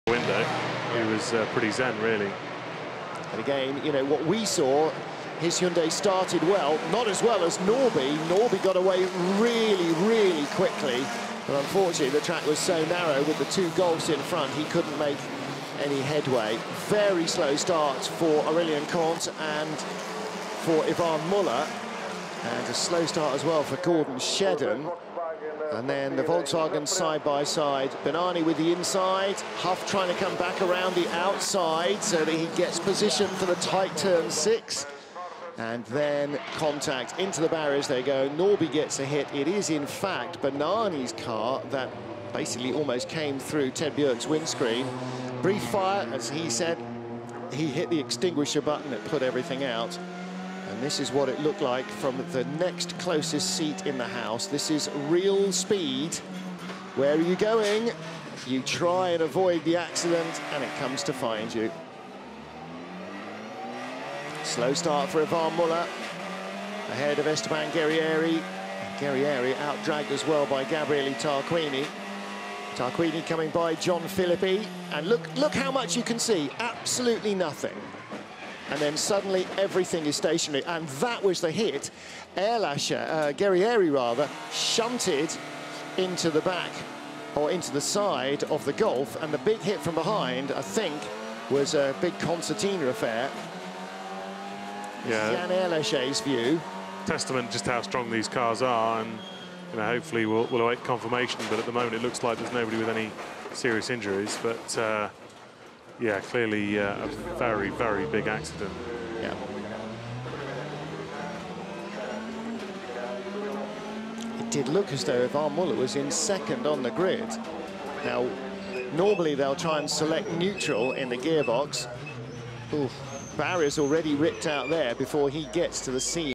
Rallye WTCR Gros crash dans les rues de vila real au Portugal
Gros accident de rallye au portugal